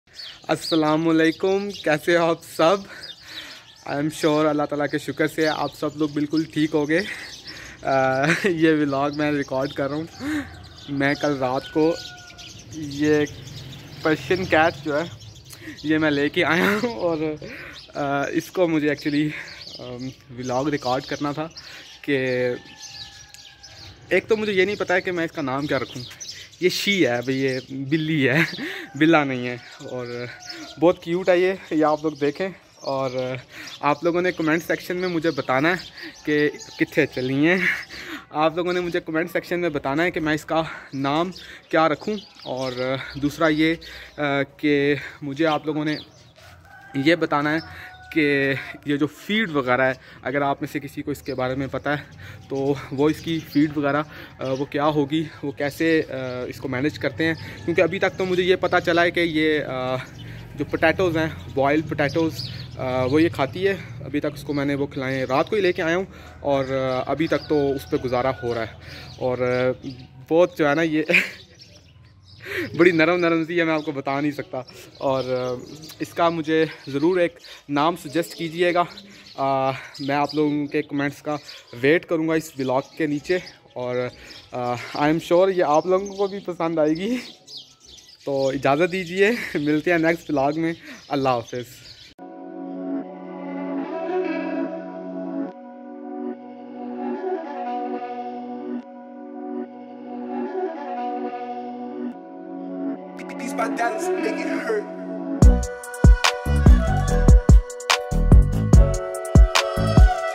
I bought a Persian Cat
Yesterday, I bought a Persian Cat. She is as polite as a pillow and  very innocent. I am sure you gonna love her also as I fell too. Do suggest a nice name for her and also some diet in the comment section. Thanks

Keep Supporting, THANKS